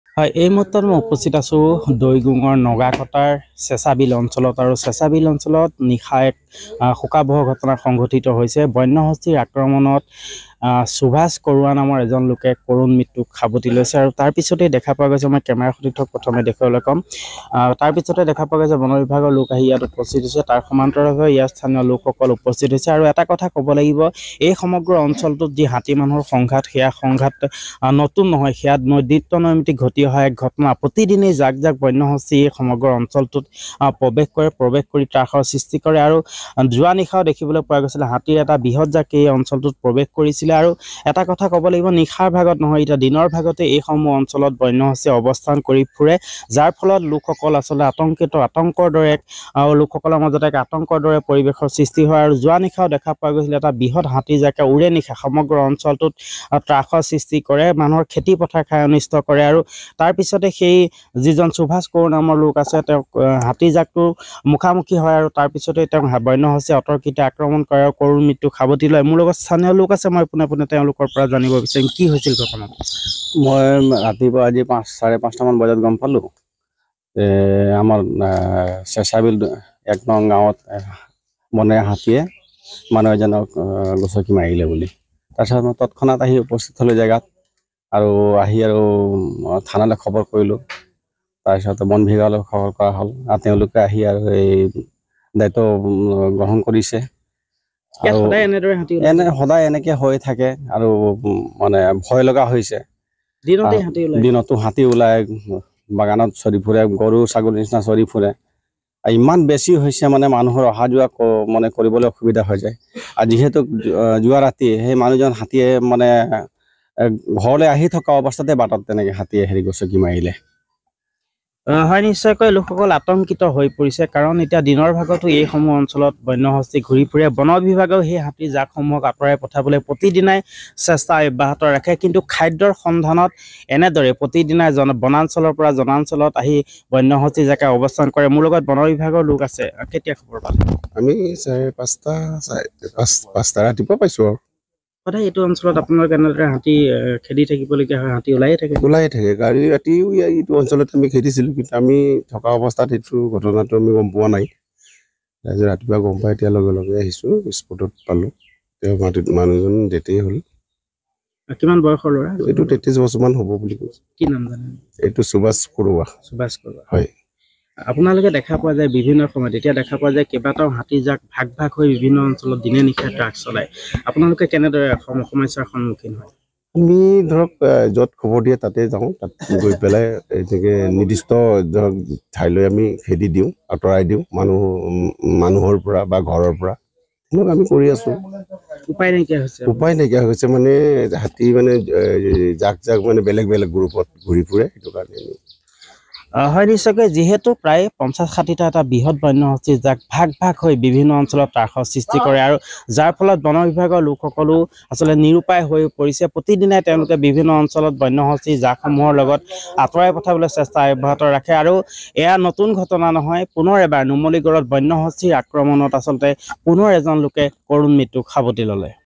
Elephant Terror In Numaligarh | নুমলীগড়ৰ ম'হখুটিত বন্যহস্তীৰ আক্ৰমণত নিহত সুভাশ কড়ৱা | N18V
লগতে চাওঁক দিনটোৰ বহু গুৰুত্বপূৰ্ণ খবৰ |